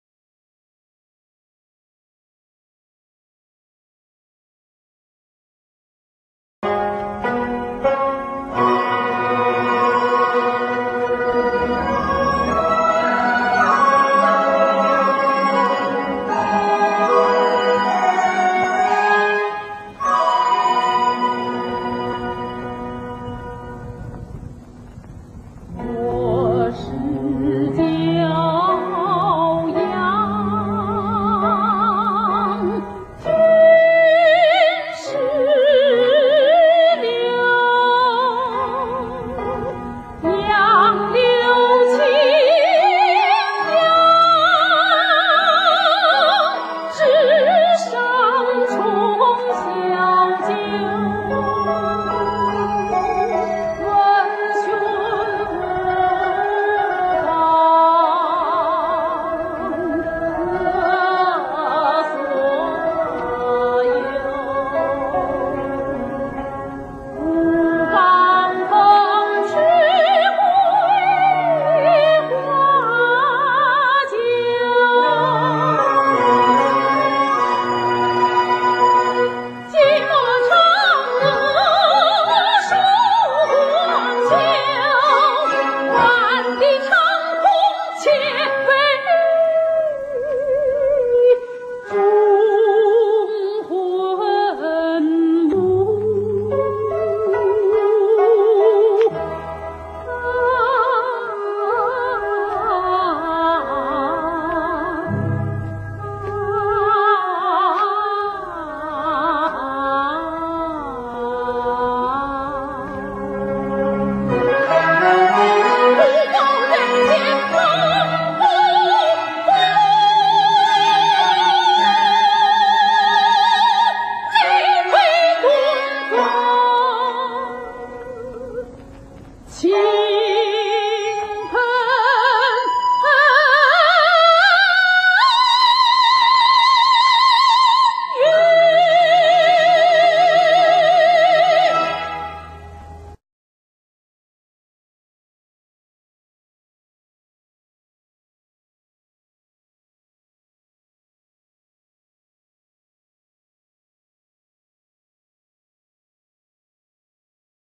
蝶恋花 -- 答李淑一 -- 毛泽东 深悼 杨开慧 -- in memory of Kaihui & Zhixun
Mao ZeDong's famous poem to deeply mourning his late wife Yang Kaihui and his comrade friend Liu Zhixun, wirrten on May 11, 1957 to Liu's wife Li Shuyi in responding to her request of a poem by Mao in the 1920's to Kaihui.